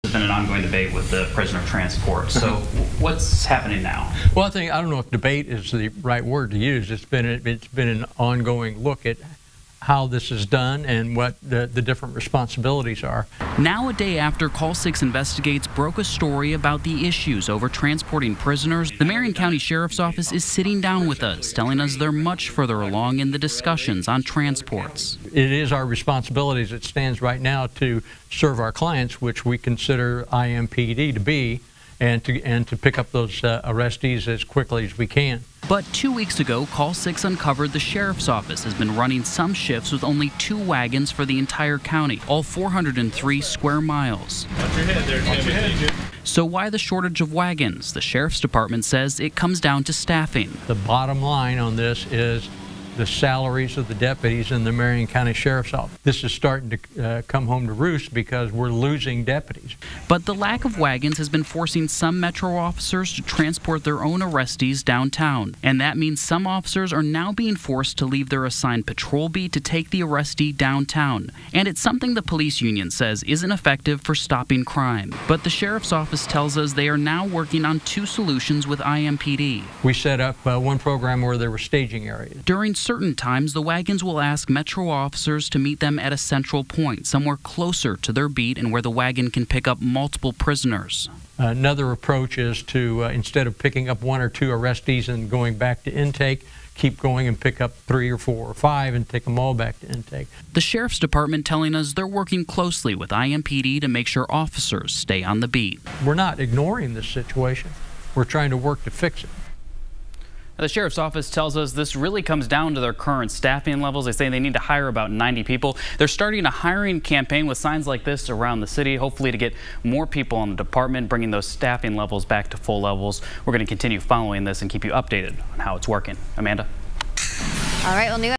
Marion County Sheriff's Office updates prisoner transport situation
The lack of transport wagons has been forcing some metro officers to transport their own arrestees downtown. That means officers have to leave their beat to take an arrestee downtown.